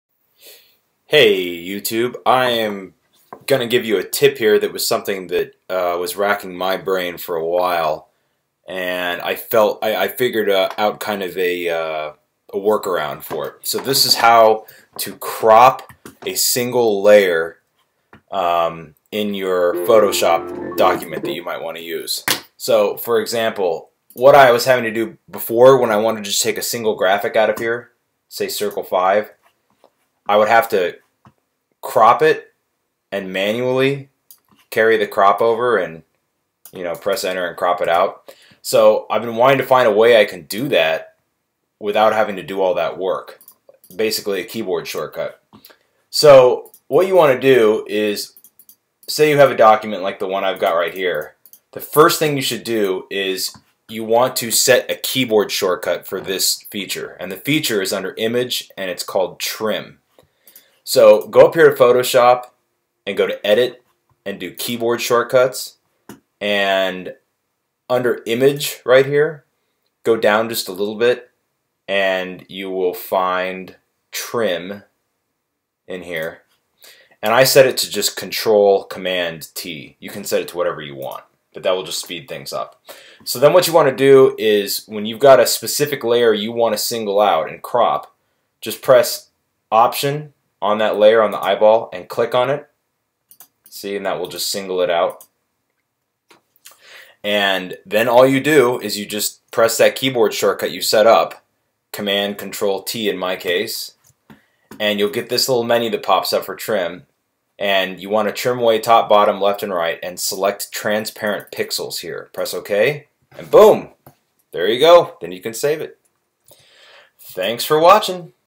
Crop or Trim to a single layer or object in Photoshop CS6
Here's a brief tutorial on how to crop to a single layer using keyboard shortcuts, saving you the time and hassle of doing it manually.